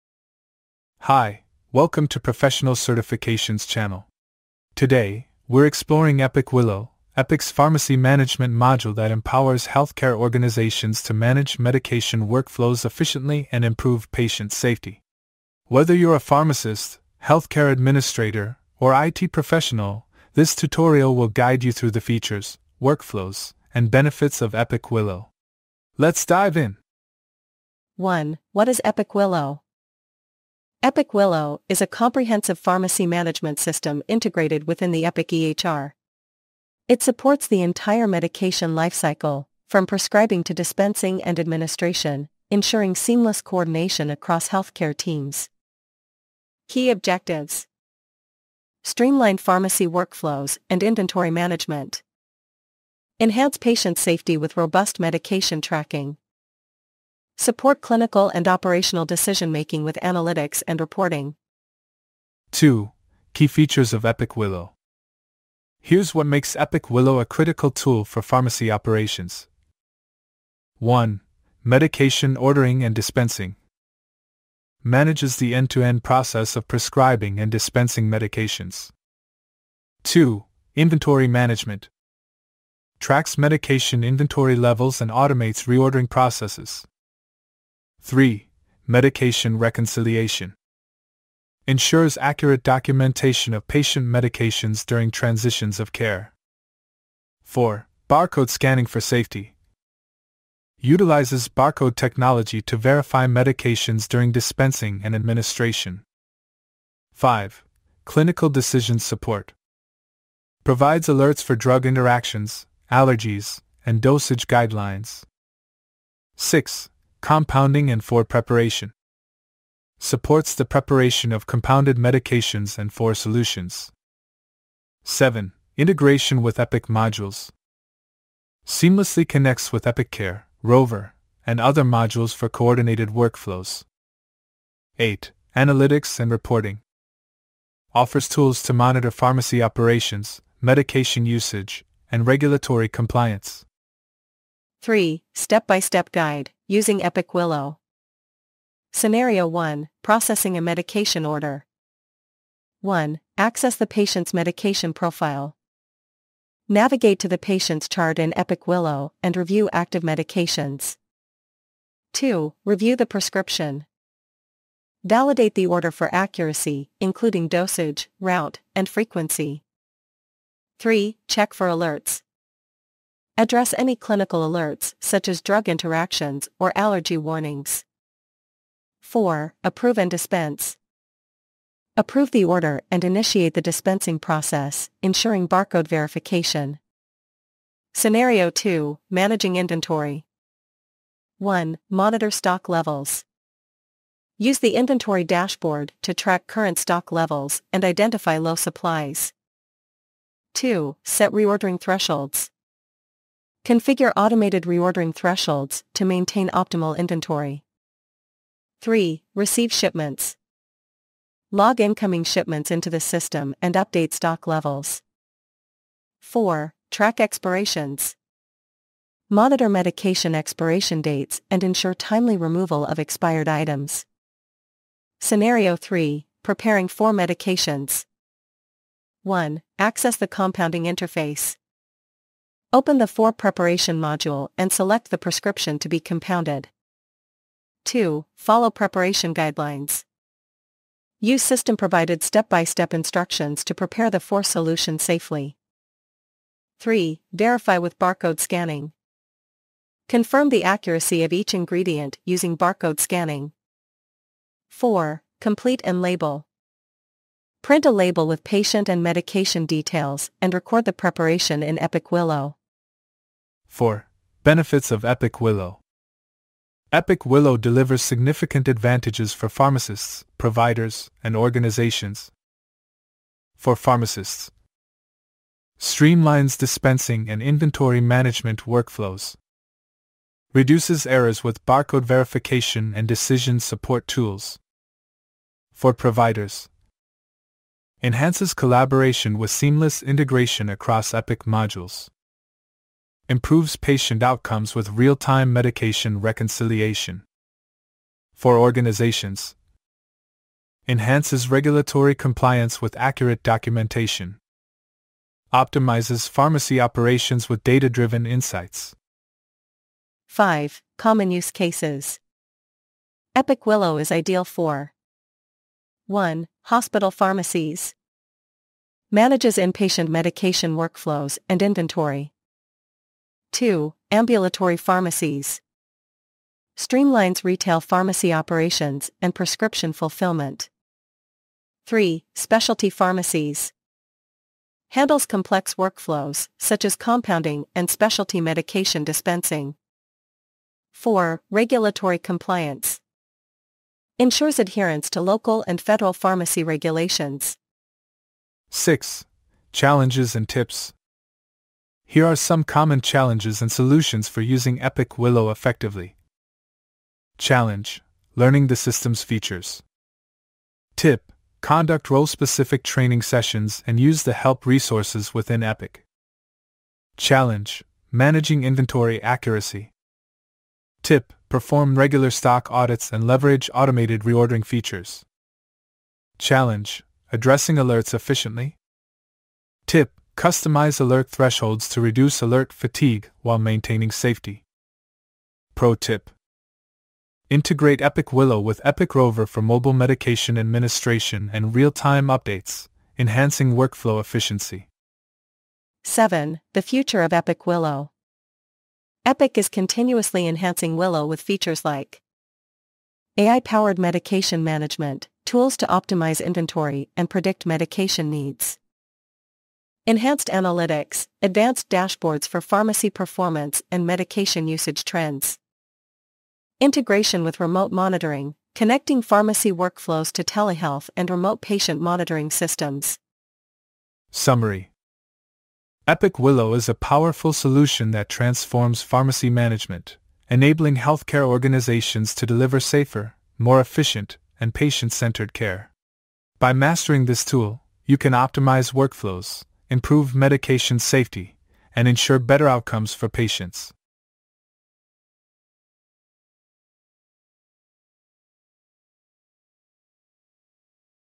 Epic Systems modules | Epic Willow
00:00   Epic Systems modules | Epic Willow |

00:27   1. What is Epic Willow?
Key Objectives:
       •   Streamline pharmacy workflows and inventory management.
       •   Enhance patient safety with robust medication tracking.
       •   Support clinical and operational decision-making with analytics and reporting.

01:02   2. Key Features of Epic Willow
       1. Medication Ordering and Dispensing:
       2. Inventory Management:
       3. Medication Reconciliation:
       4. Barcode Scanning for Safety:
       5. Clinical Decision Support:
       6. Compounding and IV Preparation:
       7. Integration with Epic Modules:
       8. Analytics and Reporting:

02:24   3. Step-by-Step Guide: Using Epic Willow
Scenario 1: Processing a Medication Order
       1. Access the Patient’s Medication Profile:
       2. Review the Prescription:
       3. Check for Alerts:
       4. Approve and Dispense:

Scenario 2: Managing Inventory
       1. Monitor Stock Levels:
       2. Set Reordering Thresholds:
       3. Receive Shipments:
       4. Track Expirations:

Scenario 3: Preparing IV Medications
       1. Access the Compounding Interface:
       2. Follow Preparation Guidelines:
       3. Verify with Barcode Scanning:
       4. Complete and Label:

04:23   4. Benefits of Epic Willow
       •   For Pharmacists:
              •   Streamlines dispensing and inventory management workflows.
              •   Reduces errors with barcode verification and decision support tools.
       •   For Providers:
              •   Enhances collaboration with seamless integration across Epic modules.
              •   Improves patient outcomes with real-time medication reconciliation.
       •   For Organizations:
              •   Enhances regulatory compliance with accurate documentation.
              •   Optimizes pharmacy operations with data-driven insights.

05:09   5. Common Use Cases
       1. Hospital Pharmacies:
       2. Ambulatory Pharmacies:
       3. Specialty Pharmacies:
       4. Regulatory Compliance:

05:48   6. Challenges and Tips
       •   Challenge: Learning the System’s Features
              •   Tip: Conduct role-specific training sessions and use the help resources within Epic.
       •   Challenge: Managing Inventory Accuracy
              •   Tip: Perform regular stock audits and leverage automated reordering features.
       •   Challenge: Addressing Alerts Efficiently
              •   Tip: Customize alert thresholds to reduce alert fatigue while maintaining safety.

Pro Tip:

06:35   7. The Future of Epic Willow
       •   AI-Powered Medication Management:
       •   Enhanced Analytics:
       •   Integration with Remote Monitoring:

07:04   Summary